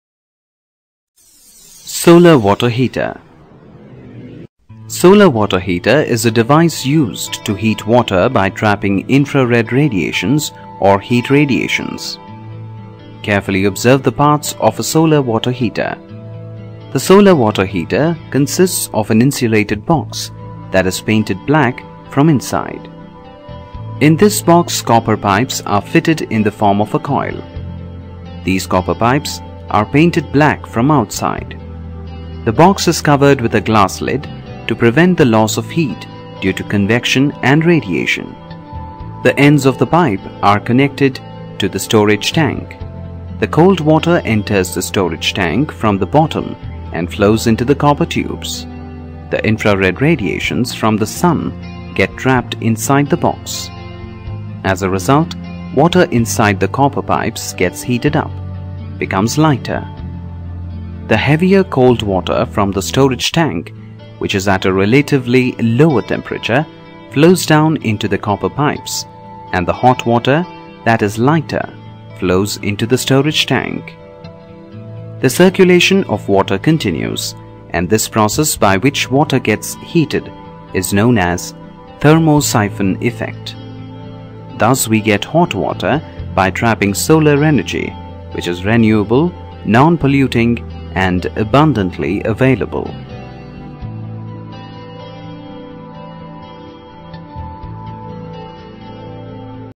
Solar Water Heater
Solar Water Heater System

Solar water heater consists of an insulated box which is painted black from inside and in which copper tubes are fitted in the form of a coil. These copper tubes are painted black from outside. The box and copper tubes of the solar water heater are painted black so that they may absorb heat rays of the sun more efficiently. The box is covered with a glass lid to prevent the loss of heat due to convection and radiation. The glass lid of the box produces green house effect in the solar water heater and traps heat rays of the sun. The two ends of the copper tube of the solar water heater are joined to a water storage tank.